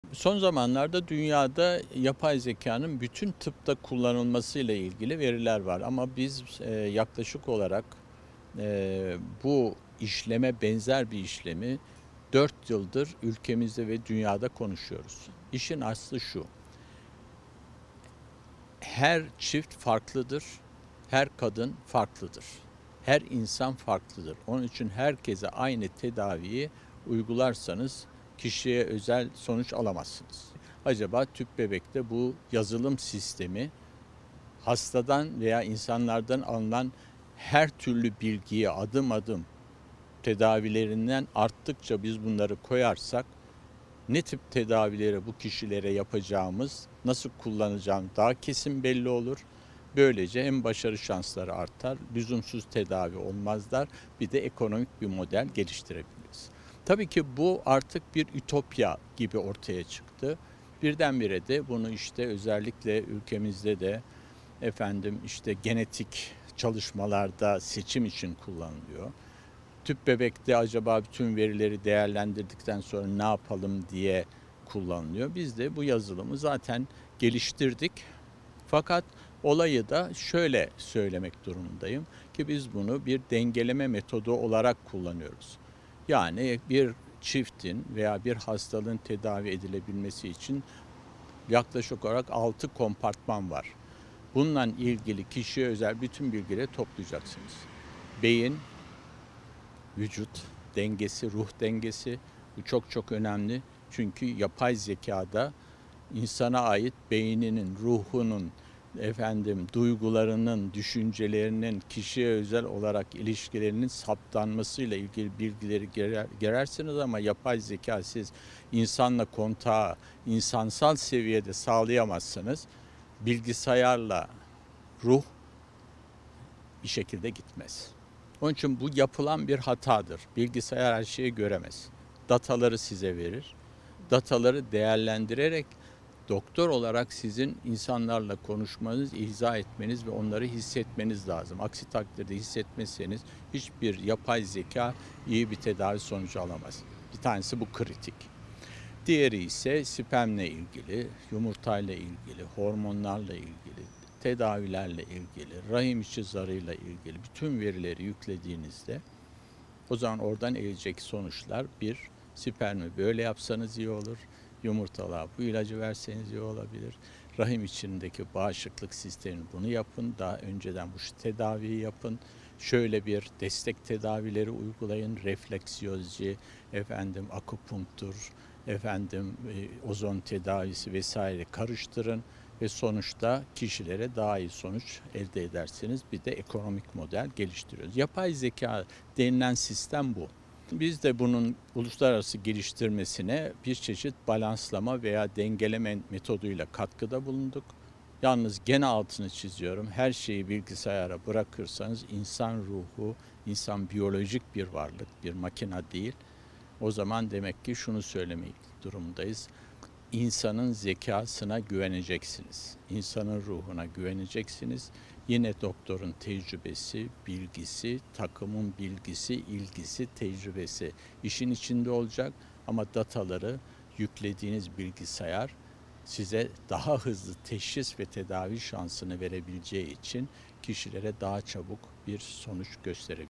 Tüp Bebek Tedavisinde Yapay Zeka Uygulaması
Tüp bebek tedavisine yapay zekanın kullanıldığına dikkat çeken Kadın Hastalıkları ve Doğum Uzmanı Prof. Dr. Timur Gürgan, yapay zekanın uluslararası geliştirilmesine dengeleme metoduyla katkıda bulunduklarını kaydetti.

 Gürgan, “Sperme, yumurta ve hormonlarla ilgili bütün verileri yüklediğinizde oradan elde edilecek sonuçlar, ‘Spermi şöyle yapsanız iyi olur. Yumurtalara şu ilacı verseniz daha iyi olur. Rahim içindeki bağışıklık sistemine bunu yapın. Şöyle bir destek tedavileri uygulayın’ bilgileri verir. Yapay zeka denilen sistem bu” dedi.
Kadın Hastalıkları ve Doğum Uzmanı ve Üreme Tıbbı ve Cerrahisi Derneği (ÜTCD) Başkanı Prof. Dr. Timur Gürgan, tüp bebek tedavisine kullanılan yapay zeka hakkında açıklamalarda bulundu. Gürgan, dünya tıbbında yapay zekanın kullanılmasıyla ilgili verilerin olduğunu kaydetti. Her insanın farklı olduğuna dikkat çeken Gürgan, herkese aynı tedavi uygulanırsa, sonuç alınamayacağının altını çizdi.
“Tıp böyle gelişiyor”
Gürgan, “Kişilere veya çiftlere ait bütün veriler bir bilgisayar sistemine yüklensin. Bir yazılım içerisine yükleyelim. Sisteme kuralım. Sistem, verilerinizi güncel bilgilerle teknolojinin yapabilecekleriyle karıştırsın ve sizin ihtiyaçlarınızın ne olduğunu size ve doktorunuza söylesin. Tıp böyle gelişiyor. Tüp bebekte bu yazılım sistemi hastalıktan veya insanlardan alınan her türlü bilgiyi adım adım koyarsak, ne tür tedavileri yapacağımız kesin belli olur. Başarı şansları olur. Bu artık bir ütopya gibi ortaya çıktı. Ülkemizde genetik çalışmalarda kullanılıyor” dedi.
“Yapay zekaya dengeleme metoduyla katkıda bulunduk”
Tüp bebek tedavisinde yapay zeka yazılımı geliştirdiklerini ifade eden Prof. Dr. Timur Gürgan şu ifadeleri kullandı:
“Biz de bu yazılımı geliştirdik. Biz bunu bir dengeleme metodu olarak kullanıyoruz. Bir çiftin ya da hastalığın tedavi edilebilmesi için yaklaşık olarak 6 kompartman var. Bununla ilgili kişiye özel bütün bilgileri toplayacaksınız. Beyin, vücut, ruh dengesi gibi. Yapay zekada insana ait beynin, ruhun, duygularının düşüncelerinin olması çok önemli. Bu yapılan bir hatadır. Bilgisayar her şeyi göremez. Dataları size verir, dataları değerlendirerek doktor olarak sizin insanlarla konuşmanız, hissetmeniz lazım. Aksi takdirde hiçbir yapay zekadan iyi bir sonuç alamazsınız. Sperme, yumurta ve hormonlarla ilgili bütün verileri yüklediğinizde oradan elde edilecek sonuçlar, ‘Spermi şöyle yapsanız iyi olur. Yumurtalara şu ilacı verseniz daha iyi olur. Rahim içindeki bağışıklık sistemine bunu yapın. Şöyle bir destek tedavileri uygulayın’ yapay zeka denilen sistem bu. Biz de bunun uluslararası geliştirmesine, dengeleme metoduyla katkıda bulunduk.”
İnsan zekasına güvenilmesi gerektiğini vurgulayan Gürgan, her şeyin bilgisayara bırakılmasının doğru olmadığını söyledi.
"Her hastalık kişiye özeldir"
Gürgan sözlerini şöyle noktaladı: "İnsanın ruhuna güveneceksiniz. Doktorun bilgisi, takımın ilgisi ve bilgisi işin içinde olacak. Dataları yüklediğiniz bilgisayar size daha iyi teşhis imkanı verecek. Sonuçta evren bir insana ihtiyaç duyacak. Beyinden bir üstün zekayı kimse yapamayacak. Her zaman insan beynine ihtiyaç olacak ama beyni daha iyi götürmek için değişik verilerle çalıştırabilirsiniz. En büyük yanlışlık insana ait bütün verileri toplamadan, teşhislere bakarak karar vermek. Her hastalık her kişide o kişiye özeldir."